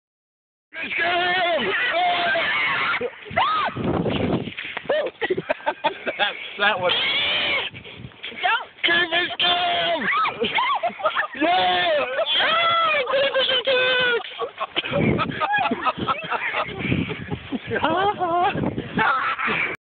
Crayfish Cam
The crayfishes point of view, and it ain't pretty.